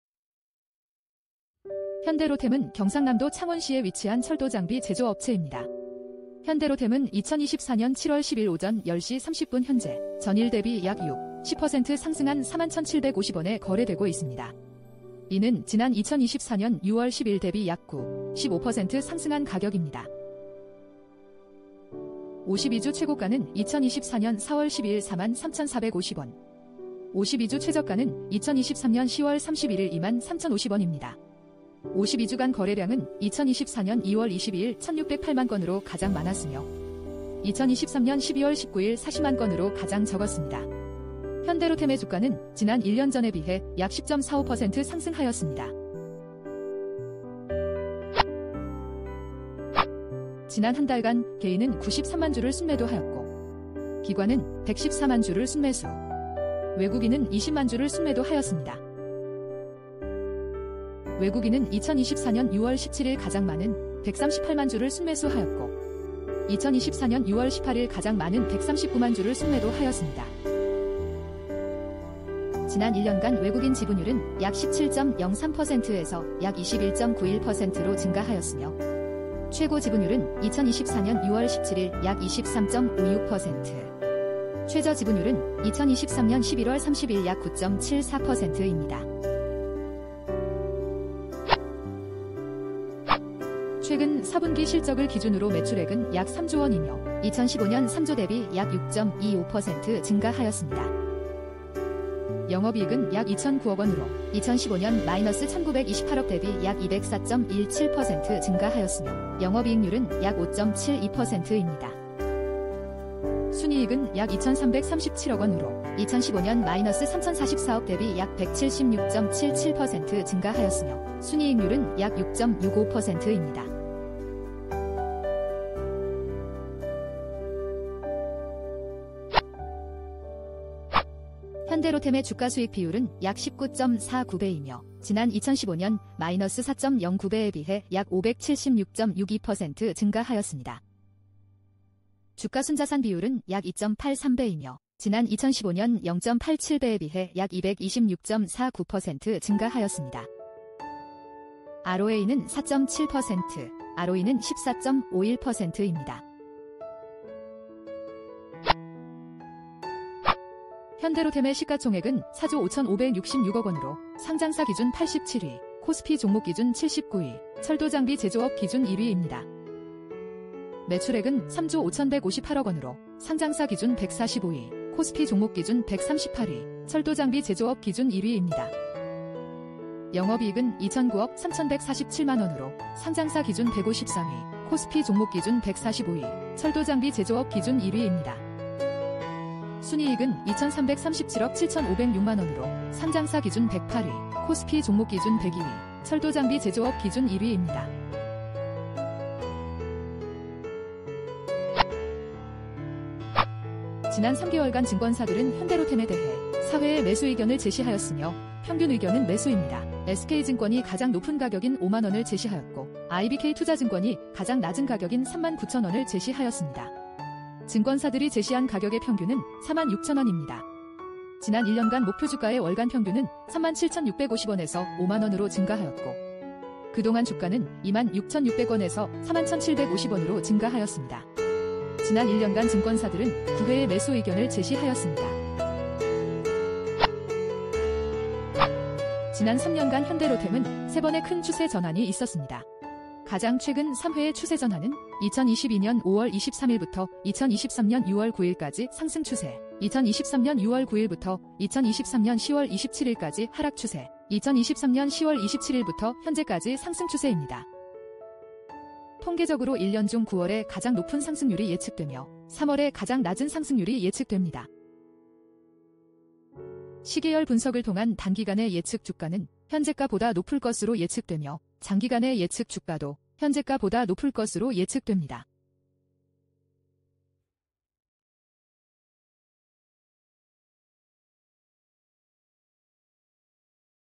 현대로템주가분석(064350) 2024년 07월 10일기준
현대로템은 경상남도 창원시에 위치한
철도장비 제조업업체 입니다.
현대로템은 2024년 07월 10일 오전 10시 30분 현재